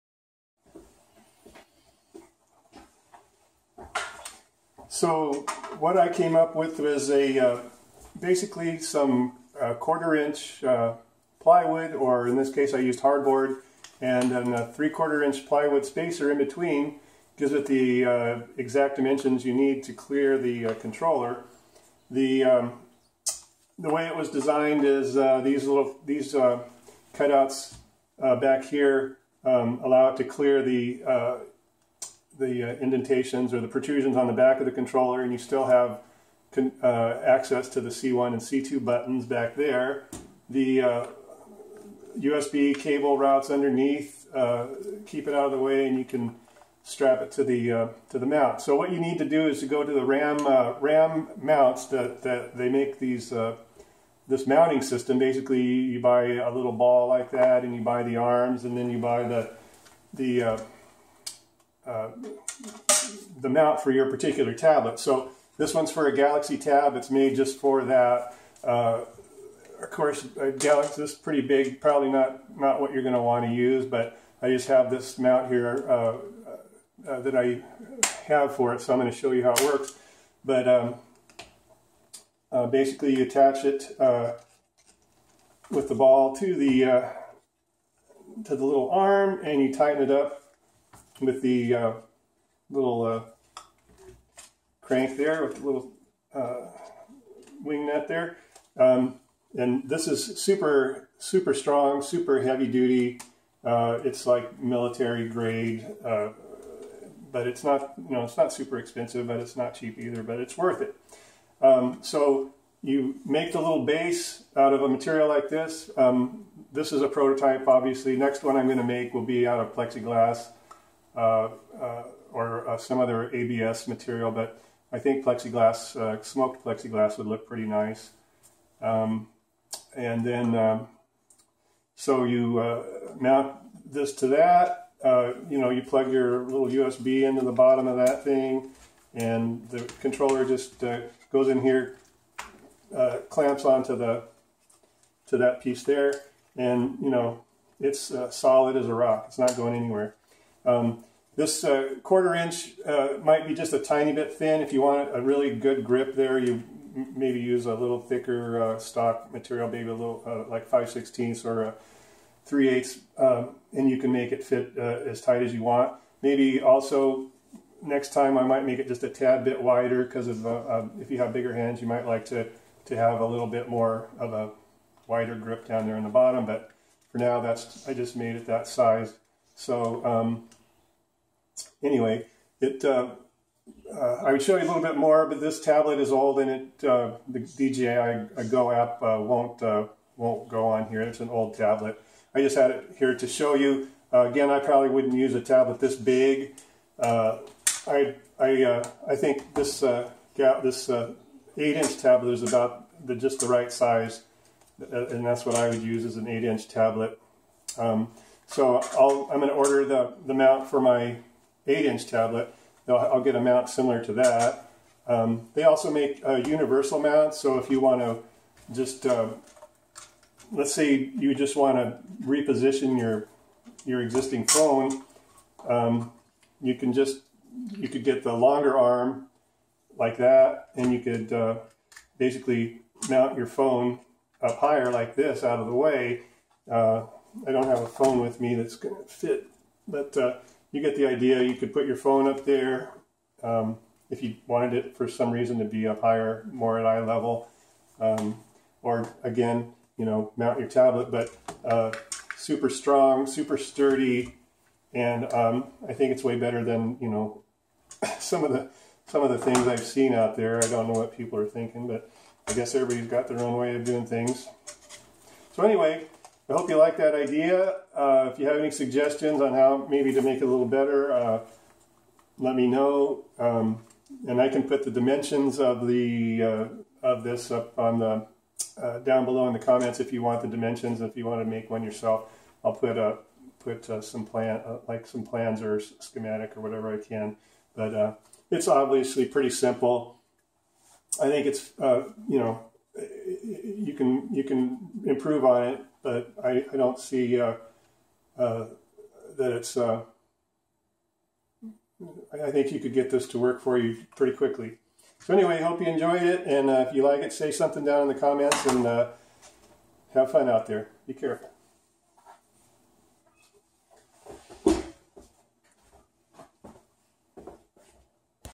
DJI Mavic Pro | Tablet Mounting Solution
Easily mount a full size tablet to your Mavic Pro controller using RAM mount accessories.